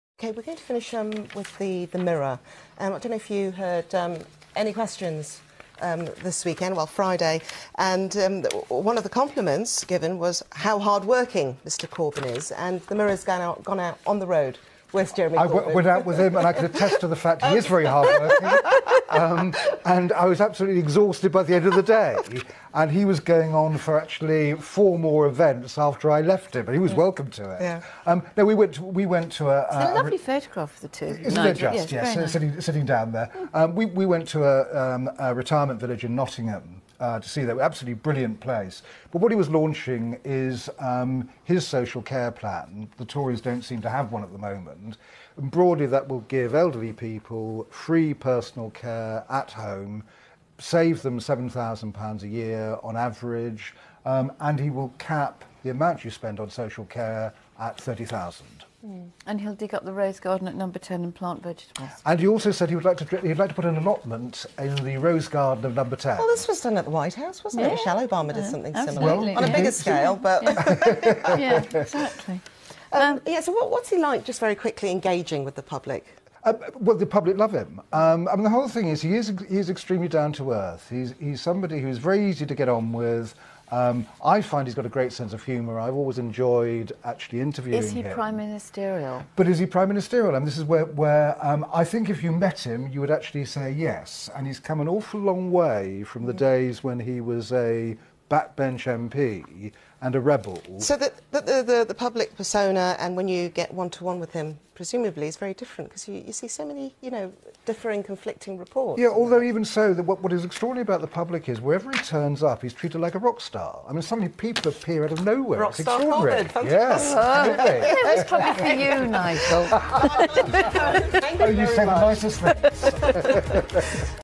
Jeremy Corbyn is "hard-working" and the public love him says BBC analyst.
BBC Analyst describes a day spent with Corbyn. He is hard-working, easy to get on with, has a great sense of humour, the public love him. "Wherever he turns up, he's treated like a rock-star".